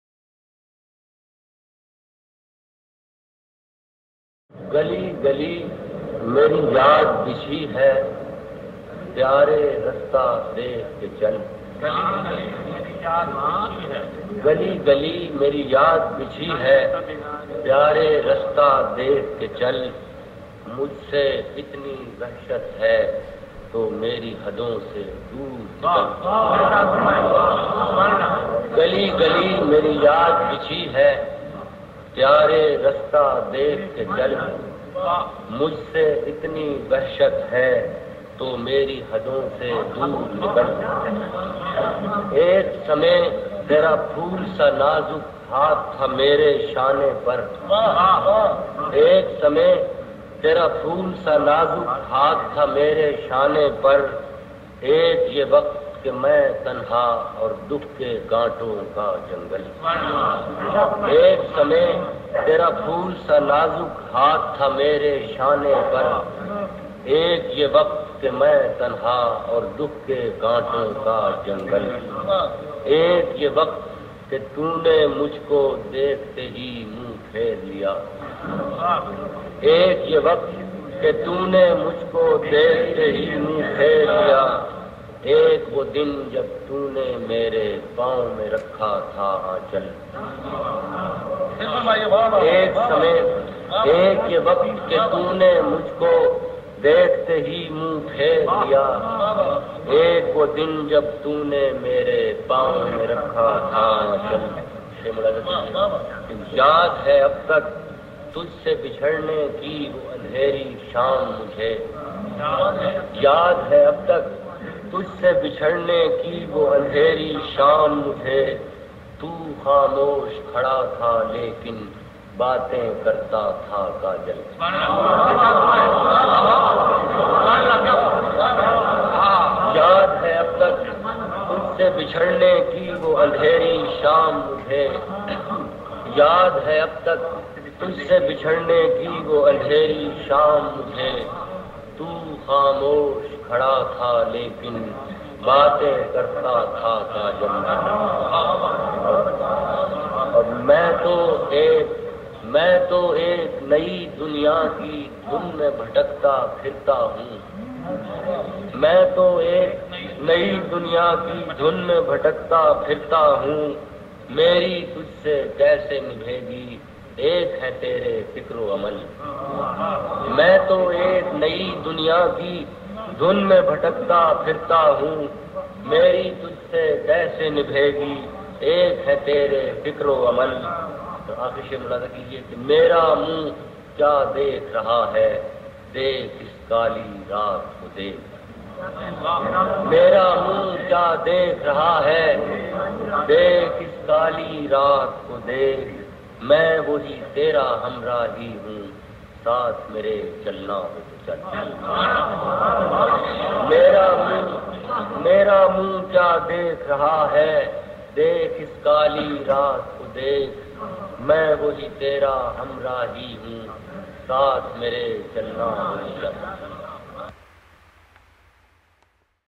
nasir kazmi: mushaira: gali gali meri yaad (ed) ناصر کاظمی: گلی گلی میری یاد
The Poet: Syed Nasir Raza Kazmi سید ناصر رضا کاظمی (takhallus: Nasir ناصر ) was born in Ambala امبالہ , Punjab (now in Haryana), British India in 1925. His father (Muhammad Sultan Kazmi محمد سلطان کاظمی ) was a Subedar Major صوبیدار میجر (a JCO, Junior Commissioned Officer) in the British Indian Army. He got his early religious education at Ambala, but then moved to Nowshehra Cantt نوشہرہ چھاؤنی, NWFP (now a Province of Pakistan), where his father was posted. When he was in fourth grade, he moved with the family to Peshawar پشاور, where his father had been posted. He stayed in Peshawar for a few years, then moved back to Ambala. He passed his Matriculation Examination (10th Grade) from Muslim High School مسلم ہائی اسکول , Ambala. For his Intermediate (12th Grade), he studied at Islamia College اسلامیہ کالج, Lahore لاہور. After completing his education from Lahore, he returned to Ambala in 1945. He migrated to Lahore, Pakistan after the creation of Pakistan in 1947. He got employed at the Radio Pakistan ریڈیو پاکستان لاہور in 1952, where he became a Staff Editor. He also worked for magazines of Urdu literature like Auraaq-e Nau اوراقِ نو and Humayun ہمایوں (of which he became the Chief Editor in 1952). He lived in Lahore where he mostly worked for the Radio. He died in Lahore on 2 March 1972. He was survived by two sons, Basir Sultan Kazmi and Hassan Sultan Kazmi۔

Nasir Kazmi started composing poetry in his teen years. Hafeez Hoshiarpuri حفیظ ہوشیارپوری was his poetry teacher (ustad استاد), who also helped him in acquiring a job at the Lahore Radio Station (where Hafeez Hoshiarpuri was the Station Director). He first started writing in the style of Akhtar Shirani اختر شیرانی (whose romantic poetry was very popular at that time). Later he was influenced by Hafeez Hoshiarpuri and Meer Taqi Meer میر تقی میر. He uses simple language and his ashaar اشعار depict the pain of a sensitive heart. His idiom is that of a common man. That made him very popular, especially among the citizens of Lahore.

In a TV interview shortly before his death, he said: "horse riding, hunting, wandering in villages, walking along the riverside, visiting mountains, were my favorite pastimes and probably this was the time when my mind got nourishment for loving nature and getting close to the expression of poetry. All my hobbies are related with fine arts, like singing, poetry, hunting, chess, love of birds, love of trees ... I started poetry because I used to reflect that all the beautiful things those I see, and those in nature are not in my hands, and they go away from me. Few moments, that time which dies, cannot be made to live. I think that it can be alive in poetry, that is why I started poetry."

His publications:
برگِ نے Barg-e Ney (1952), سُر کی چھایا Sur Ki Chhaya (1971, Verse Drama), دیوان Deewaan (1972, ghazals), پہلی بارش Pehli Baarish  (1975, ghazals), نشاطِ خواب Nishat-e Khwab (1977, poems/nazms), خُشک چشمے کے کنارے Khushk Chashmey ke Kinarey  (1982 , Prose: essays, interviews, etc), ناصر کاظمی کی ڈائری Nasir Kazmi Ki Dairy (1995), کلیات Kulliyaat. He also edited selected poetry of Meer Taqi Meer, Nazeer Akbarabadi, Wali Deccani, and Insha Ullah Khan Insha.

خاموش تماشائی
KhamoshTamashai